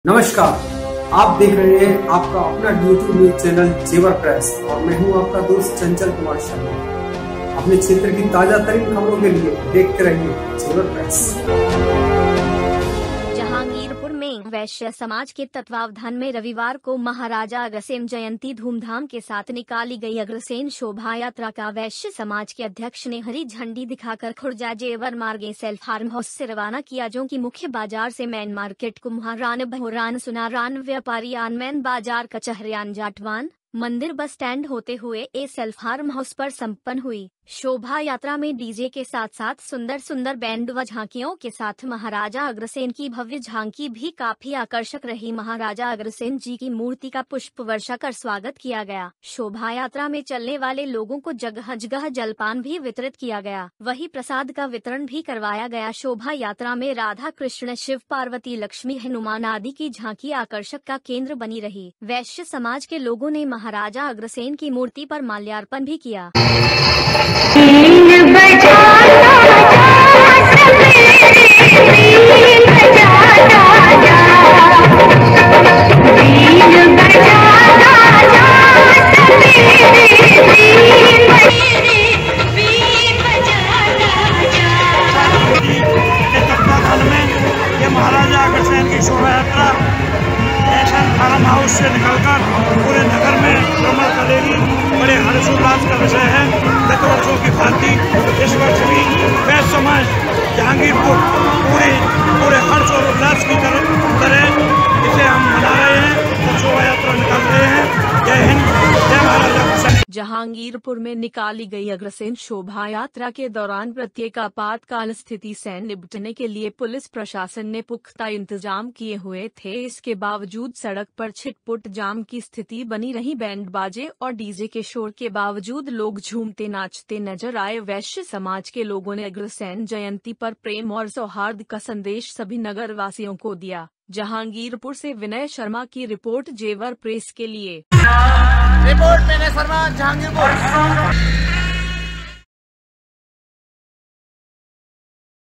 Jewar press-जहाँगीरपुर मे महाराज अग्रसेन शोभा यात्रा
जहाँगीरपुर मे हर्षोल्लाश के साथ निकली गयी महाराज अग्रसेन की शोभा यात्रा